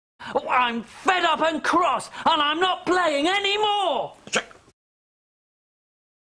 my reaction that i did not go to the entertainer toyshop today
that means i won’t be coming back to town known as the nottingham city centre again